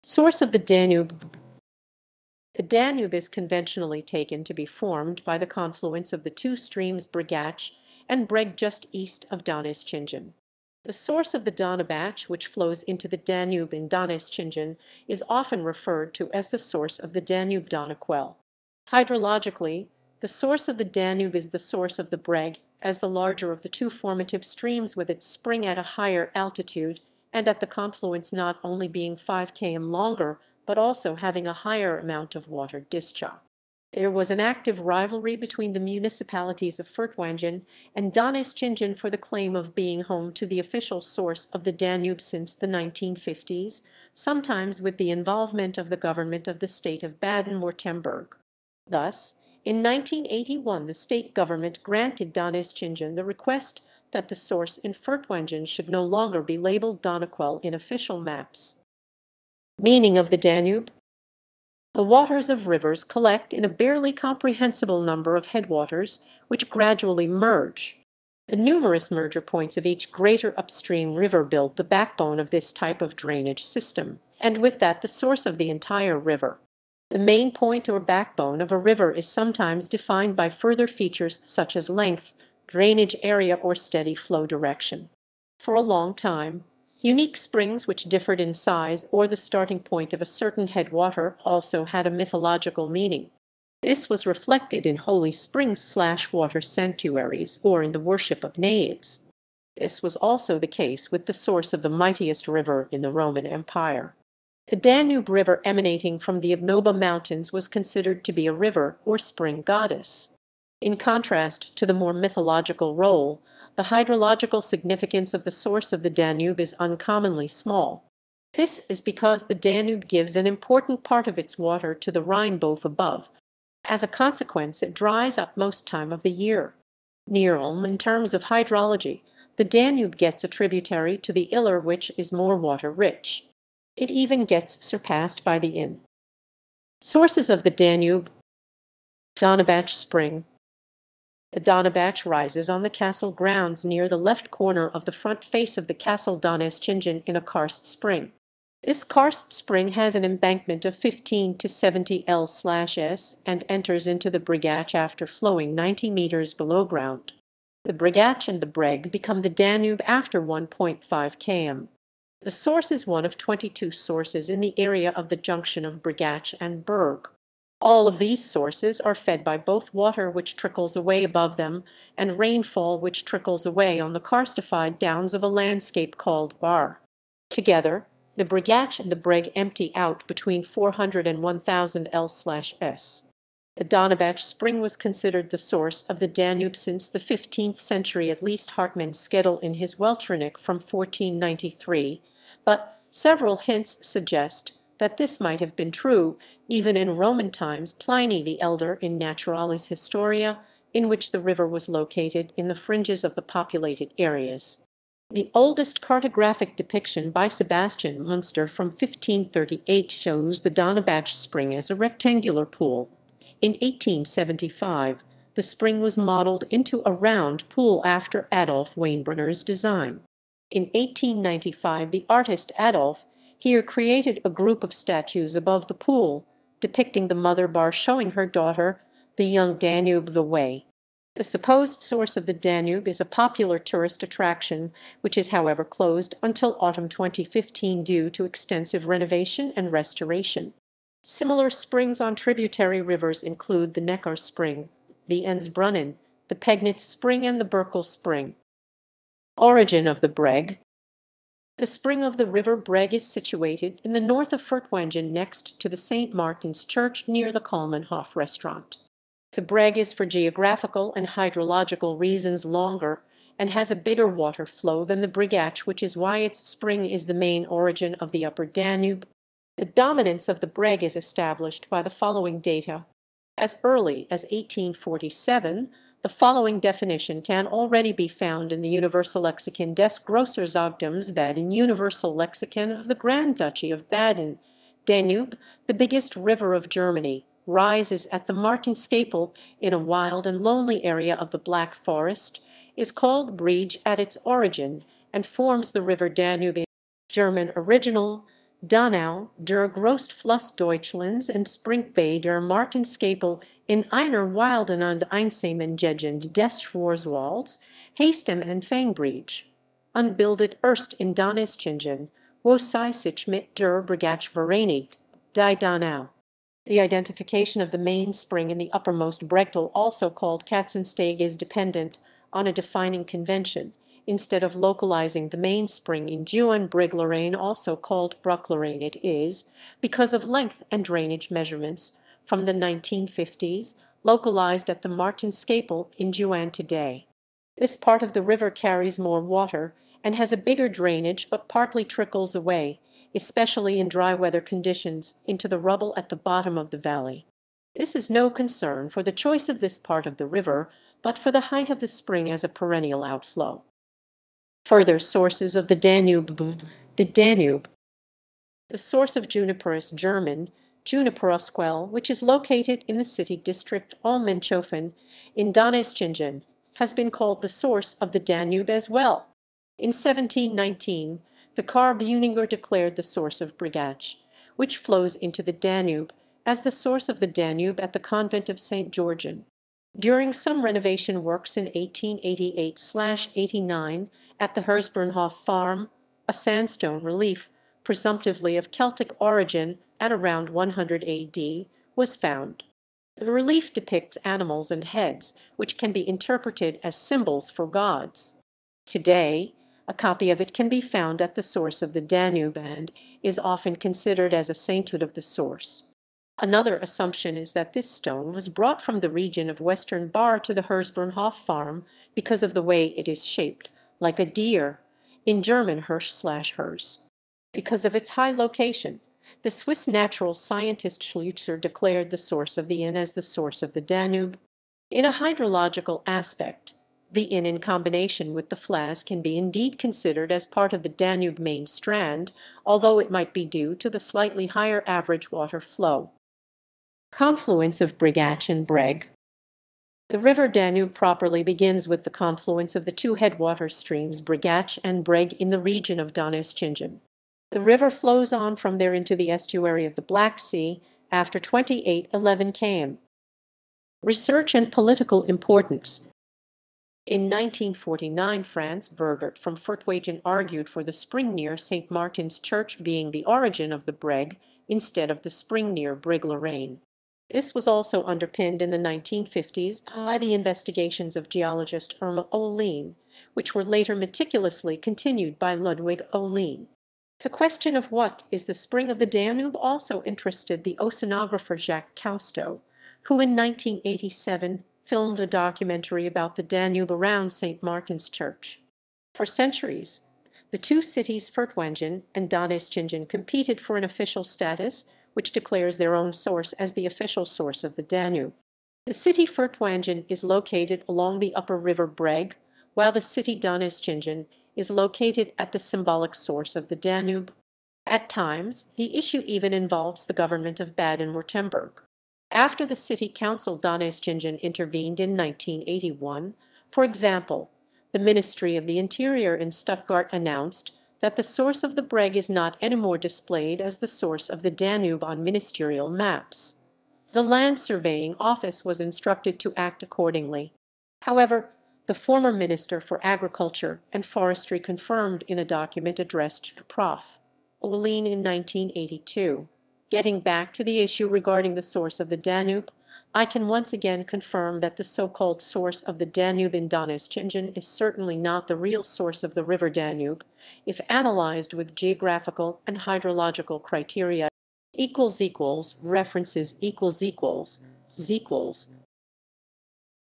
Source of the Danube
The Danube is conventionally taken to be formed by the confluence of the two streams  Brigach and Breg just east of Donaueschingen.
The source of the Donaubach, which flows into the Danube, in Donaueschingen is often referred to as the source of the Danube (Donauquelle). Hydrologically, the source of the Danube is the source of the Breg as the larger of the two formative streams (with its spring at a higher altitude and - at the confluence - not only being 5 km longer but also having a higher amount of water discharge) which rises  near Furtwangen.
There was an active rivalry between the  municipalities of Furtwangen and Donaueschingen for the claim of being home to the "official" source of the Danube since the 1950s, sometimes with the involvement of the government of the state of Baden-Württemberg.
Thus, in 1981 the state government granted Donaueschingen the request that the source in Furtwangen should no longer be labelled Donauquelle in official maps.